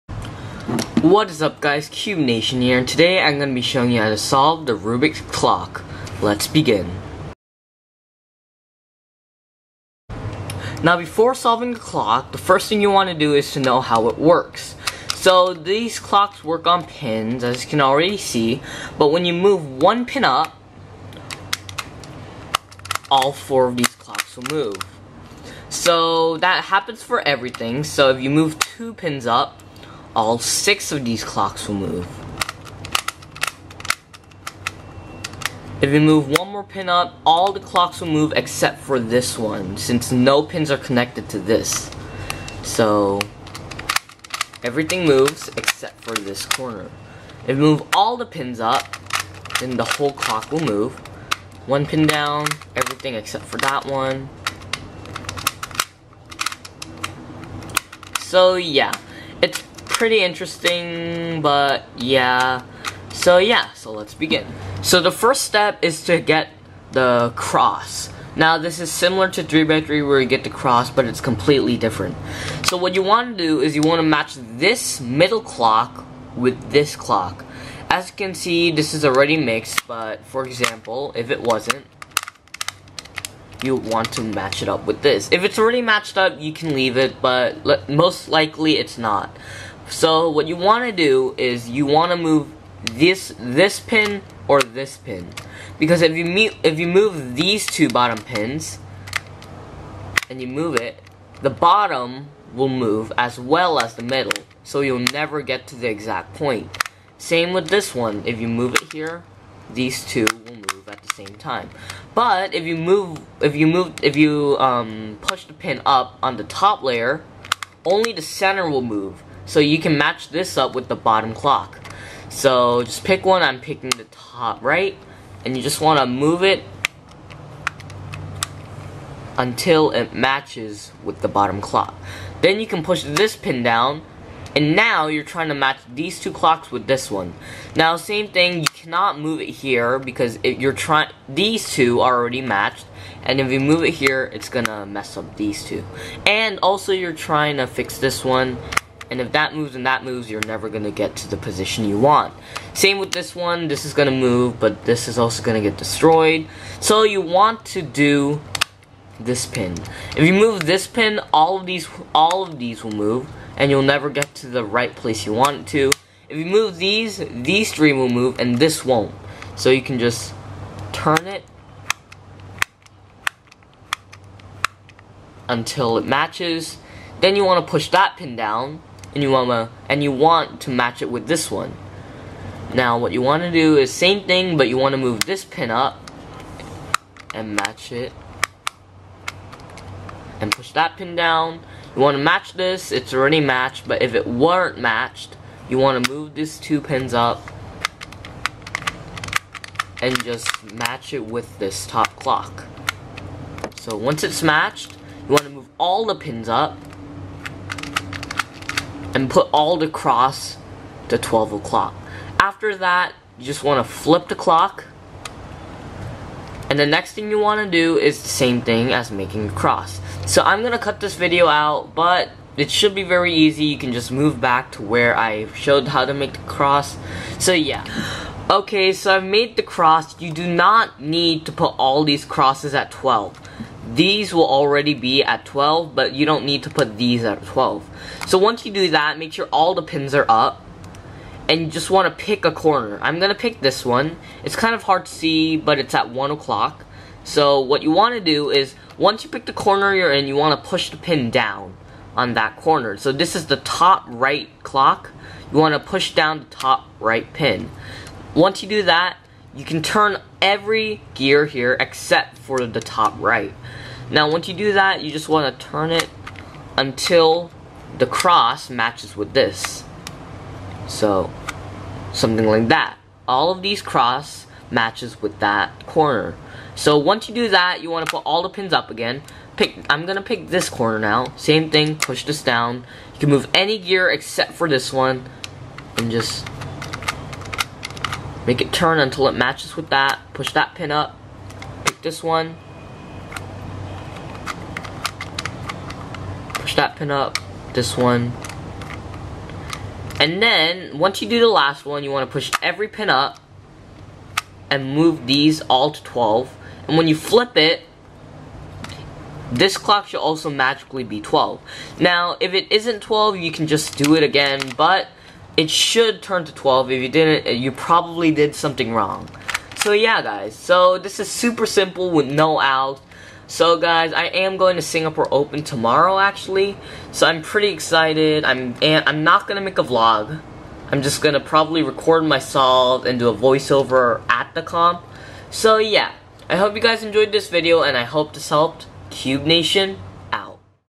How to Solve a Rubik's Clock
Cube Nation

My Clock's plastic has been broken, so I apologize for that, and please let me know if the clock was hard to see because of the lighting.

“Our Whole life is solving puzzles”
Erno Rubik.

Join the Nation, Cube Nation.